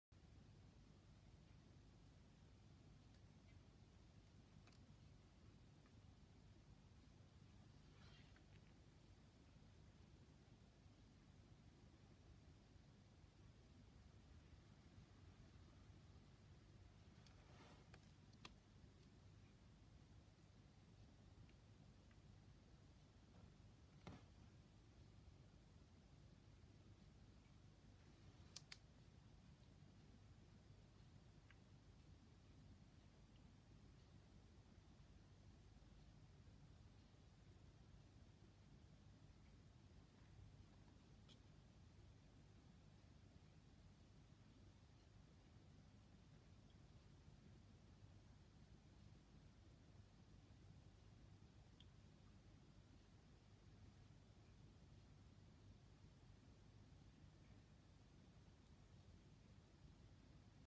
再アップ・夢を見てケイレンする子猫たち 生後11日め 20160517 Kittens to convulsions while watching the dream
Kittens to convulsions while watching the dream (11 days after birth)

昨日アップしたものが投稿エラーのため、再アップです。
すみません…

脳が発達する時期だからかな？ 眠りながら夢を見ているらしく、ピクピクと痙攣していることがよくあります。
ボケ担当のキジトラちゃんは時々目を開けたまま寝ていて、ちょっと怖い（笑